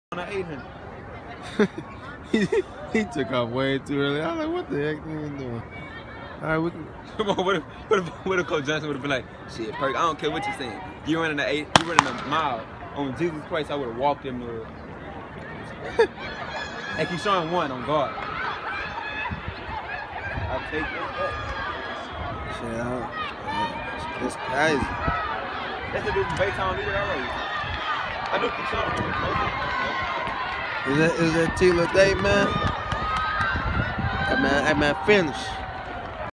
Keshawn Morris 200 m dash (blue shirt, blue shorts) c/o '19
Ft. Tyler Laday (c/o '18)
Dremar Washington (c/o '21)